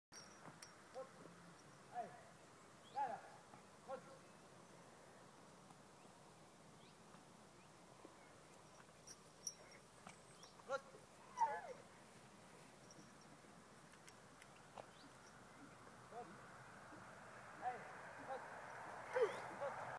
Socialisation de luminoso
On tente de rendre sa vie de cheval a Luminoso. Sa fait des bisous tout mignon avec Voltéo ! -3